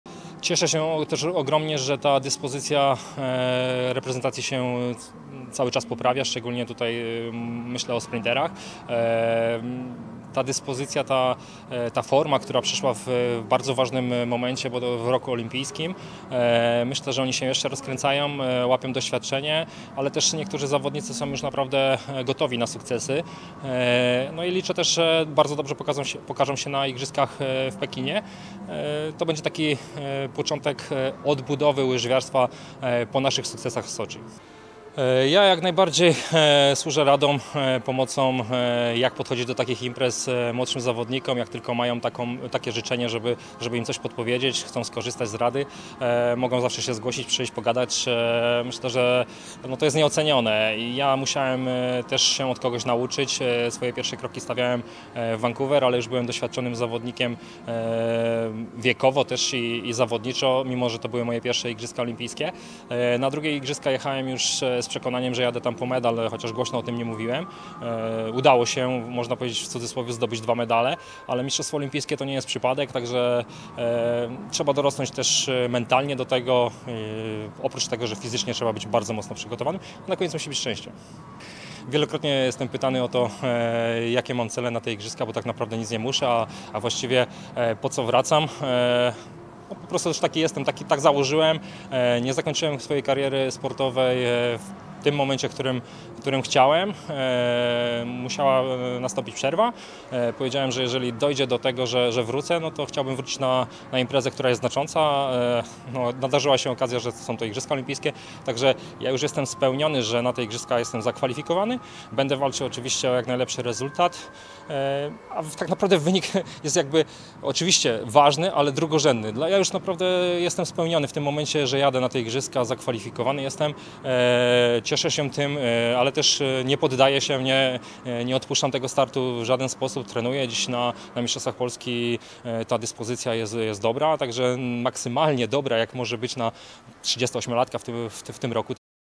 Zbigniew Bródka, łyżwiarz szybki ruszył do Pekinu na ZIO
57-osobowa reprezentacja Polski wyleciała na XXIV Zimowe Igrzyska Olimpijskie w Pekinie. Biało-Czerwoni będą rywalizować o medale w 10 konkurencjach. Ich zmagania ze stolicy Chin będą relacjonować dziennikarze Polskiego Radia.

Mistrz olimpijski z Soczi będzie jednym z chorążych reprezentacji podczas ceremonii otwarcia. Jaki cel ma spełniony już panczenista? „Jak na 38-latka jestem w formie”.